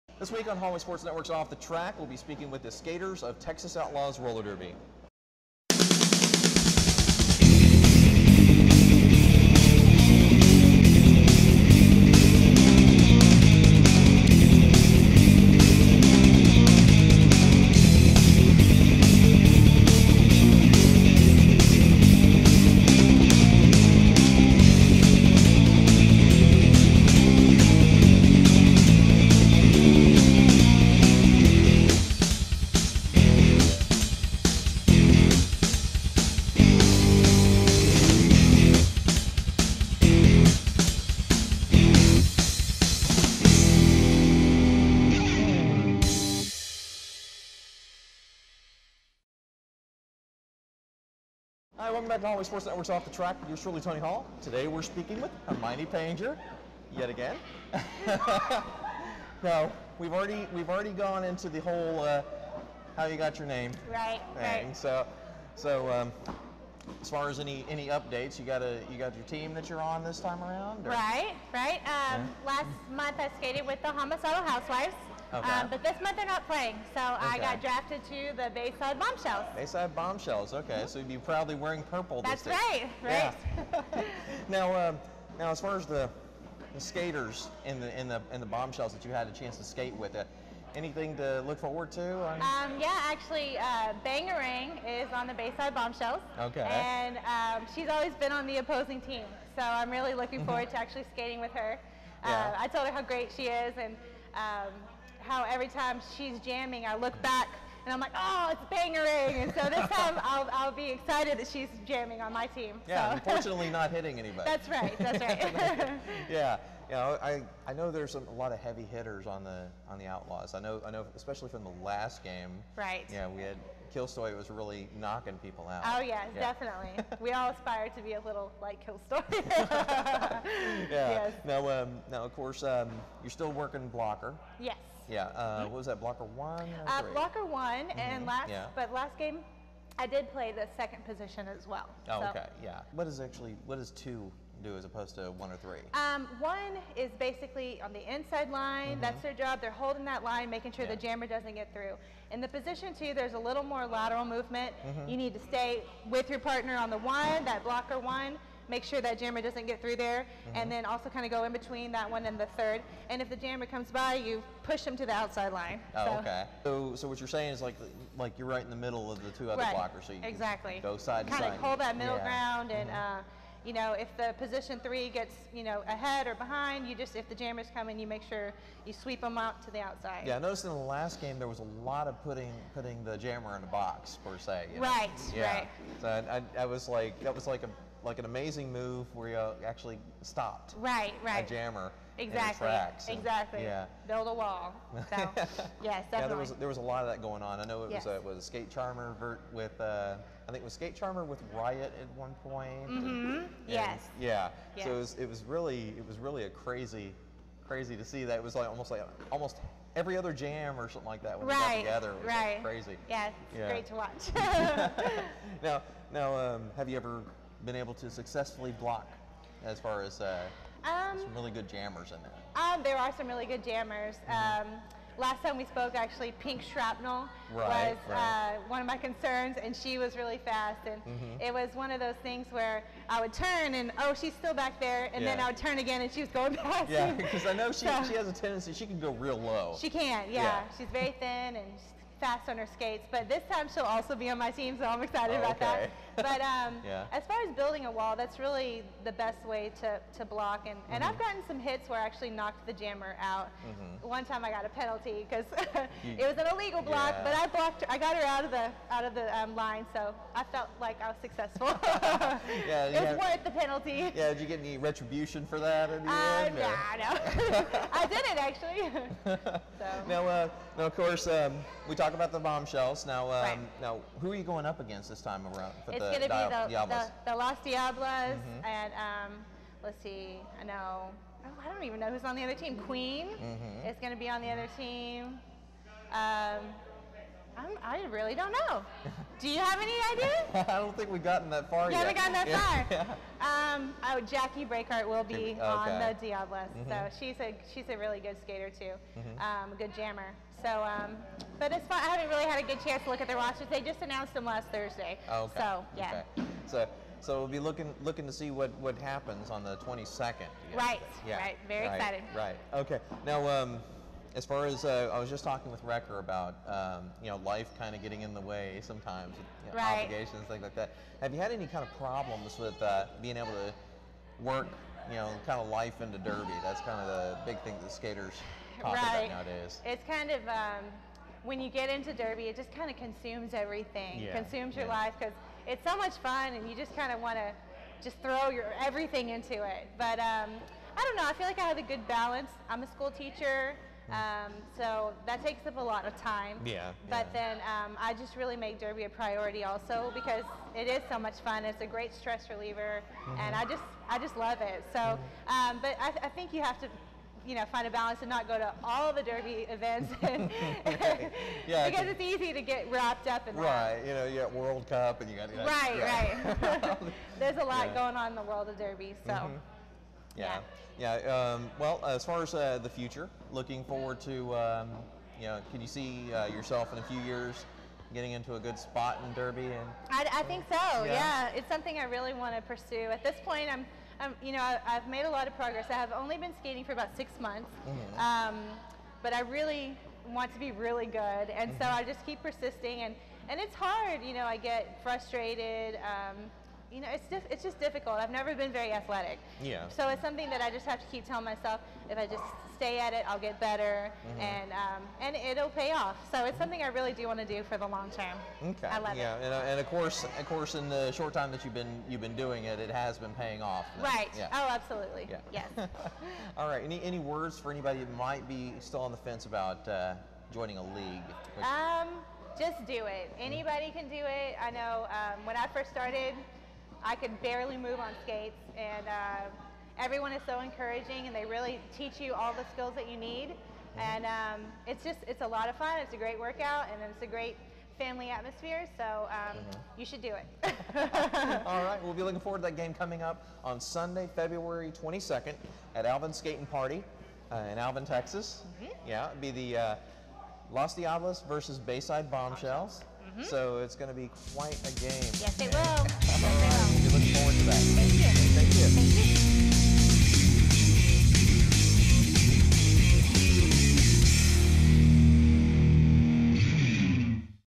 HSN_Off The Track - Interview With Hermione Painger
Tony Hall sits down to talk about some important topics involving the sport of Roller Derby with local rollergirls and officials.

Featured Skater - Hermione Painger / Texas Outlaws Roller Derby League

Description